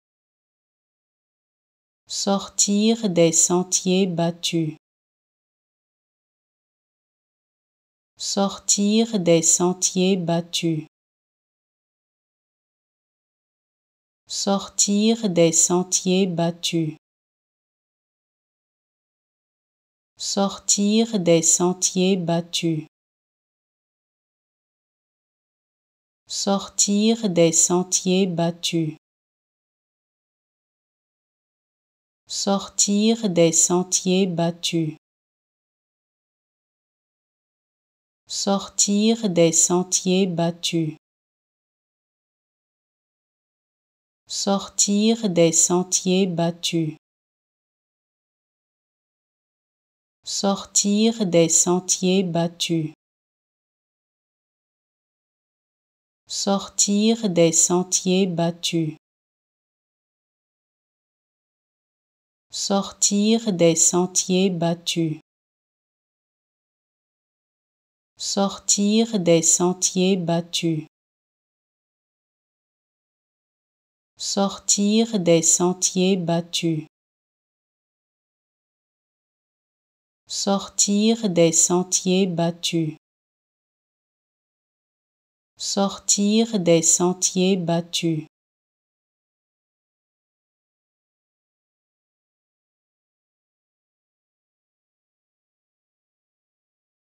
How to Pronounce ''Sortir des sentiers battus'' (Go off the beaten track) Correctly in French
Learn how to say and properly pronounce ''Sortir des sentiers battus'' in French with this free pronunciation tutorial.